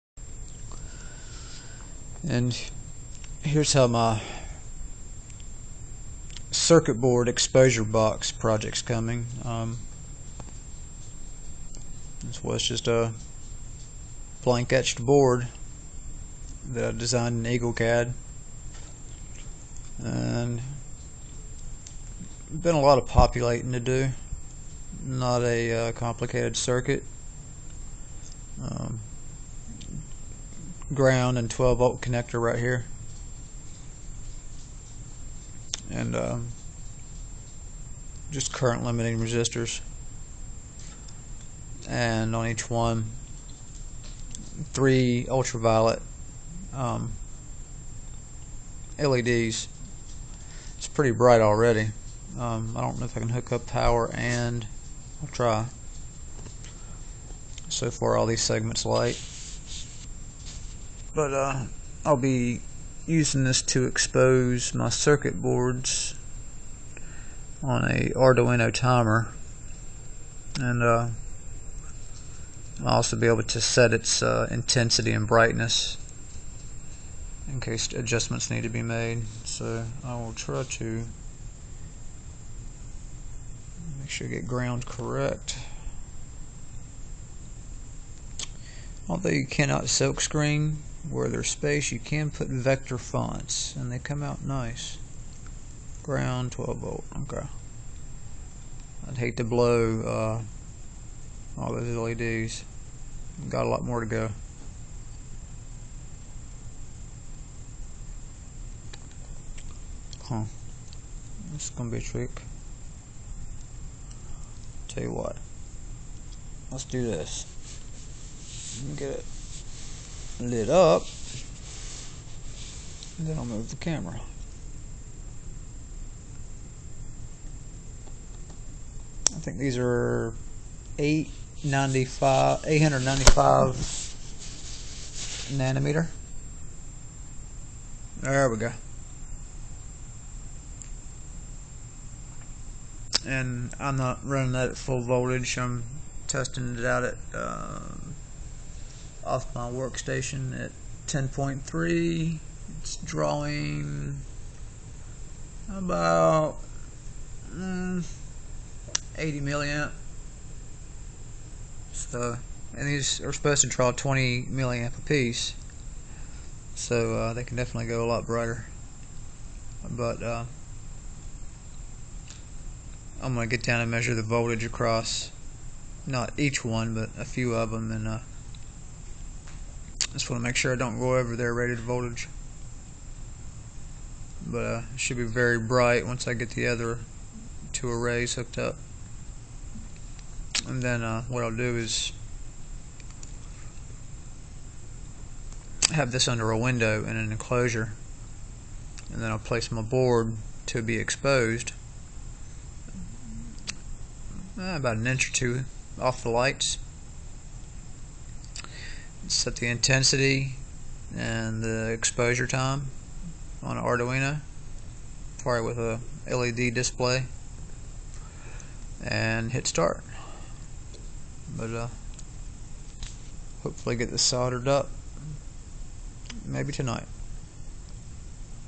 DIY Circuit Board Exposure Box Using UV LED's
Exposure board almost done.  Intend on using along side an Arduino Nano to be used as a timer and to control light intensity.  In the end will have an additional light board for exposing 2 layer PCB's